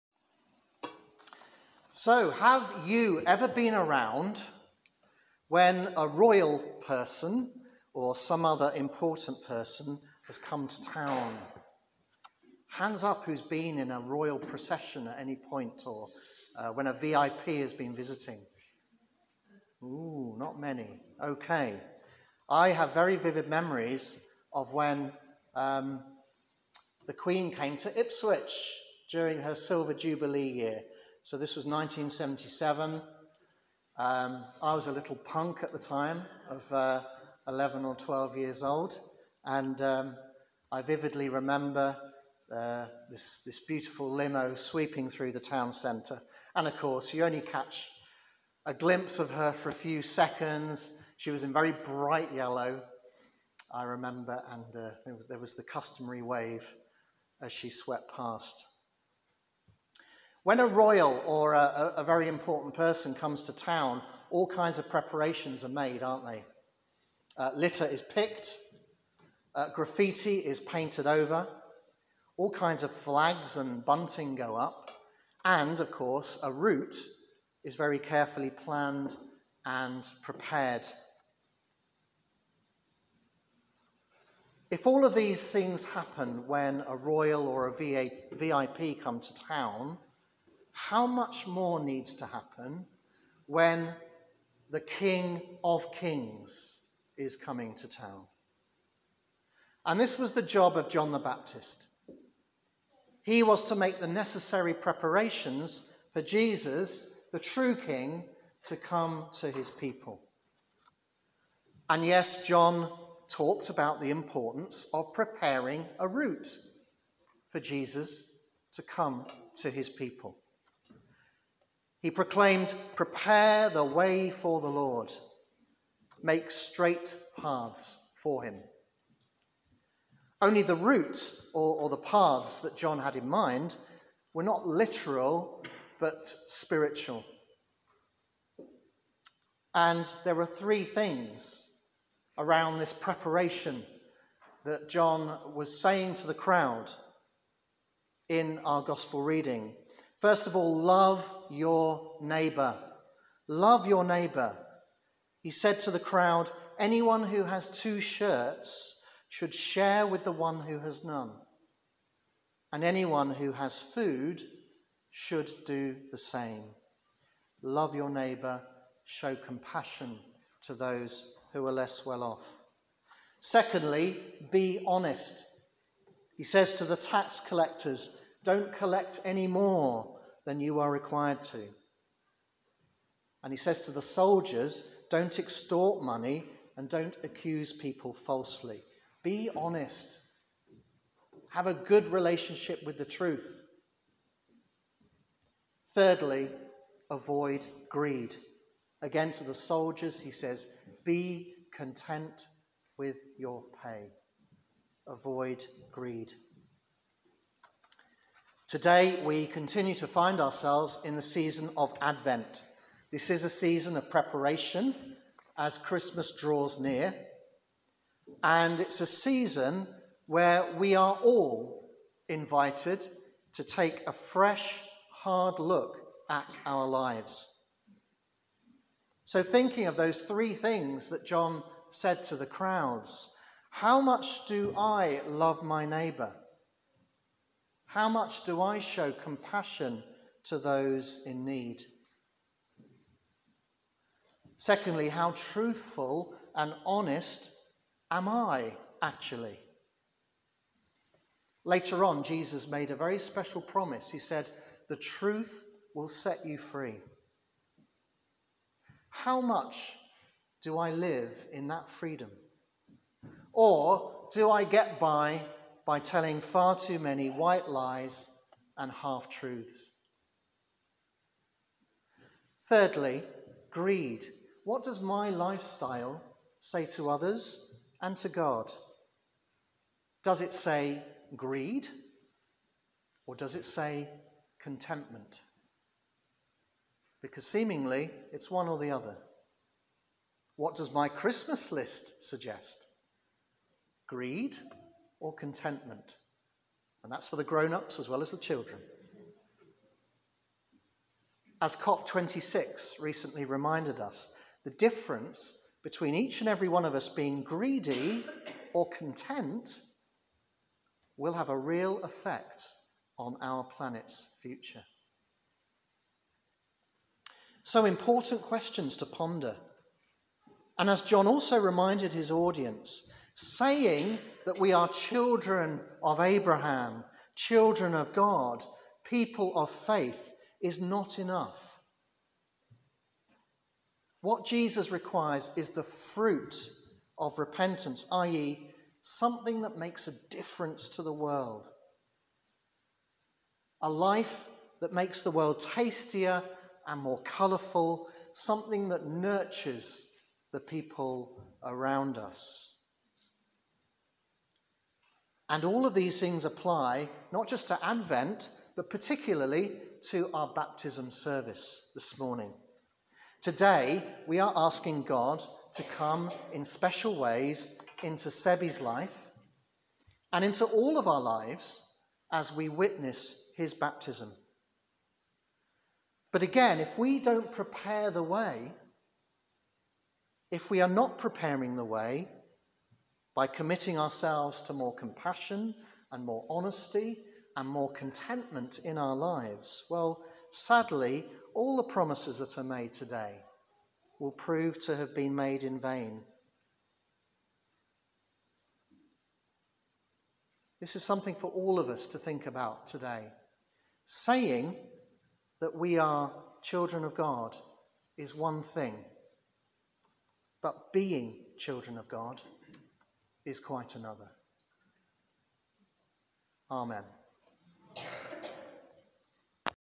Prepare The Way Of The Lord- A Sermon for Advent 2
Rev'd. Andrew Bradley shares a sermon from the 10.30 service at St. Wilfrid's Northenden on 5th December 2021.